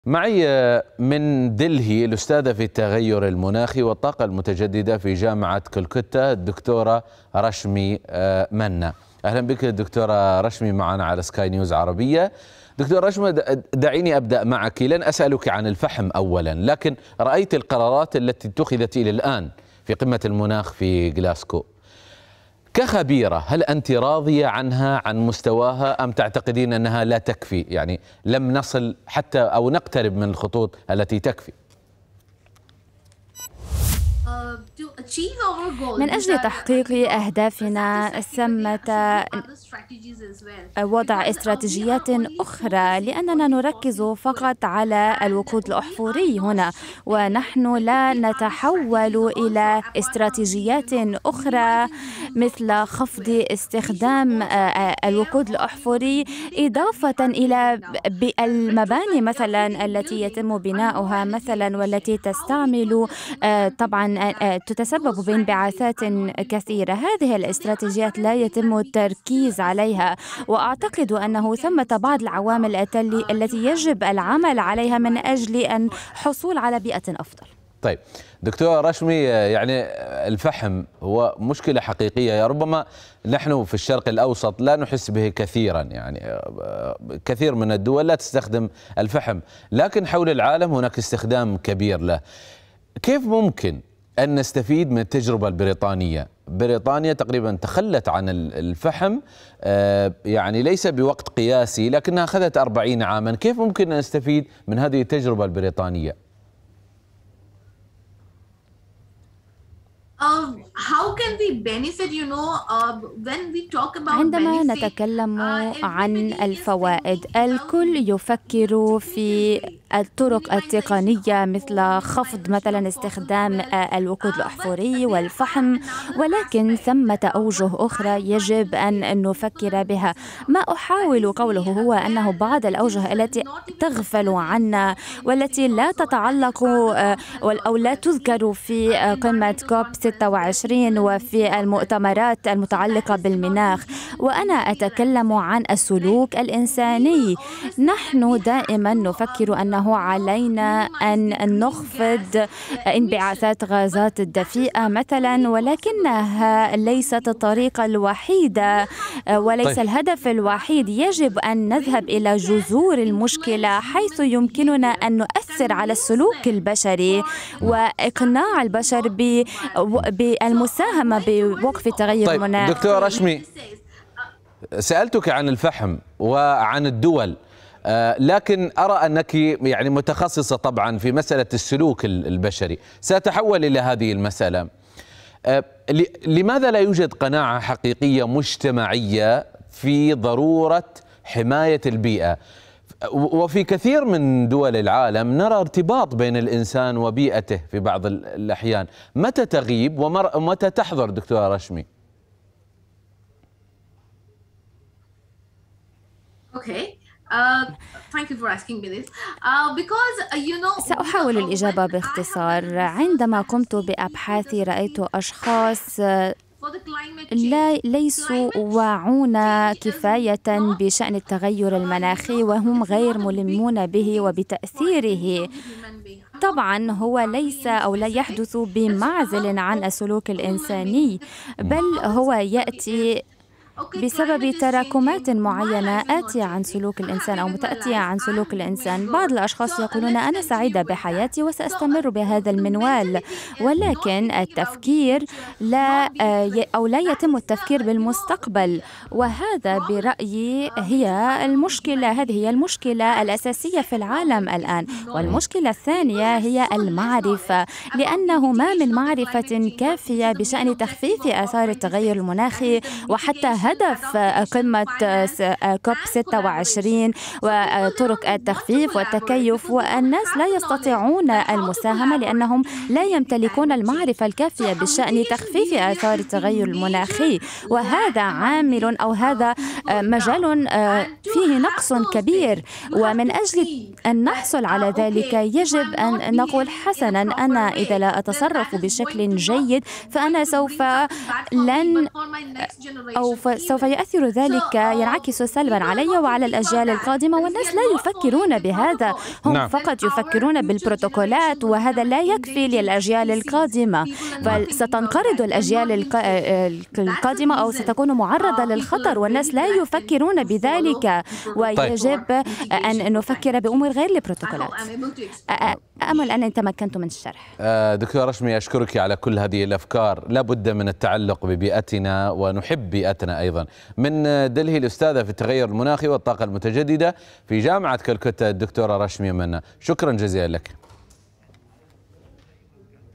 الفحم يشكل أكبر مصدر مستقل لتوليد الكهرباء
الفحم، ملِكُ الثورة الصناعية، والدعامةُ الرئيسية لتوليد الكهرباء لأكثرَ من قرن، أمام مرحلة جديدة، يتوجه فيها العالمُ لإنهاء الاعتماد عليه، إنقاذا للمُناخ من الانبعاثات..
معنا من دلهي، الأستاذة في التغير المناخي والطاقة المتجددة في جامعة كلكوتا  الدكتورة رشمي منا.